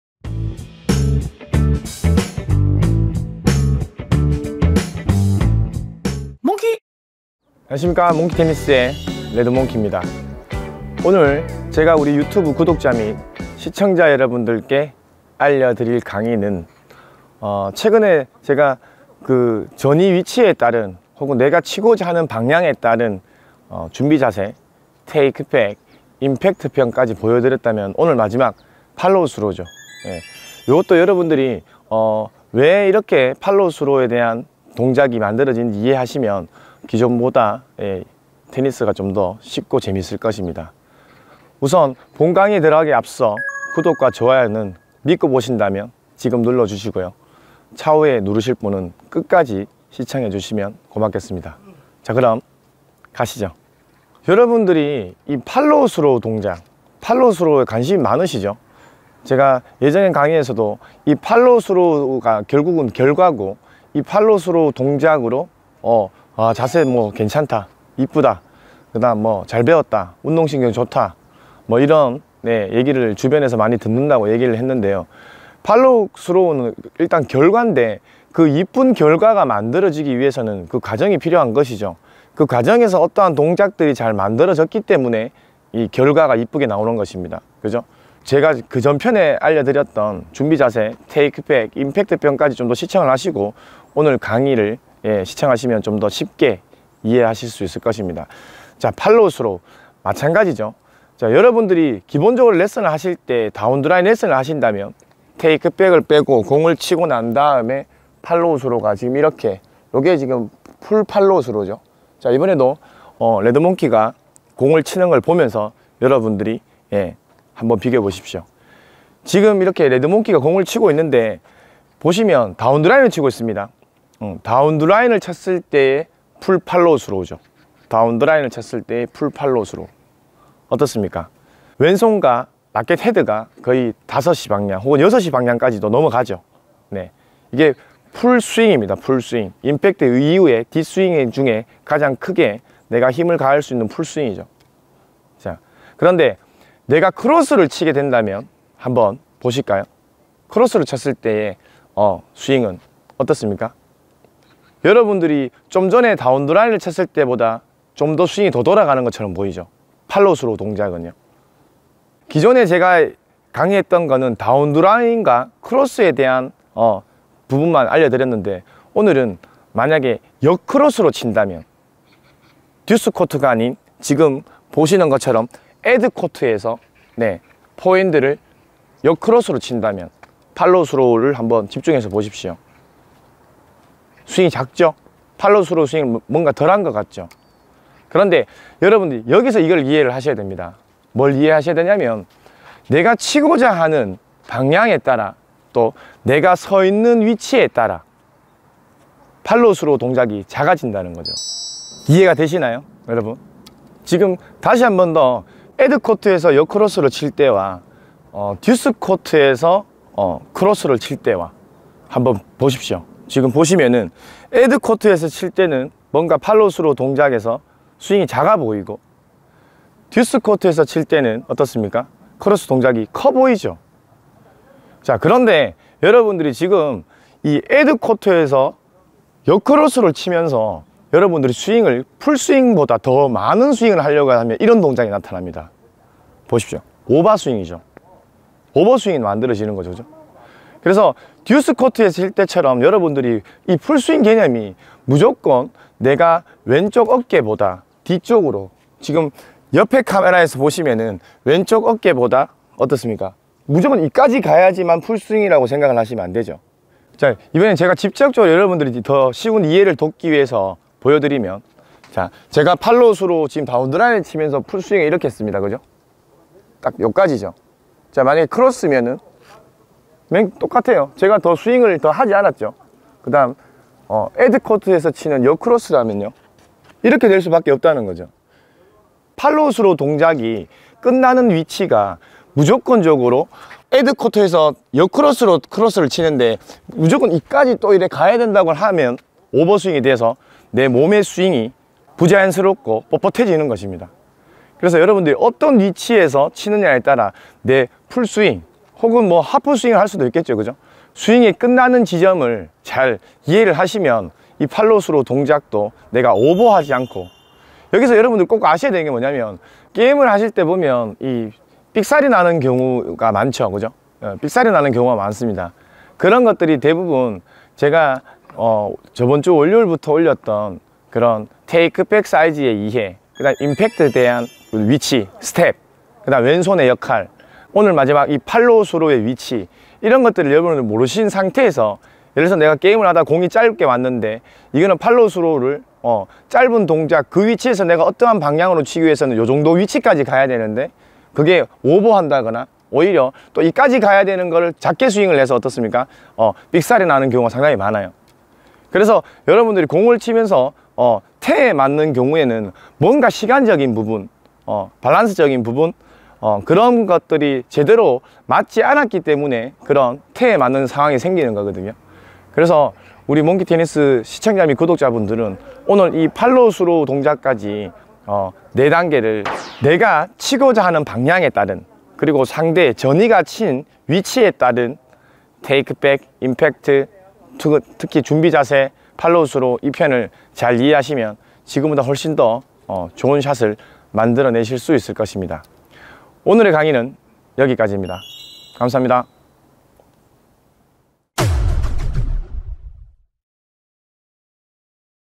테니스포핸드-멋진자세를만들수있는팔로우스루(내맘대로방향4부)
오늘은 포핸드팔로우스루에 대한 강의입니다.

감사합니다^^;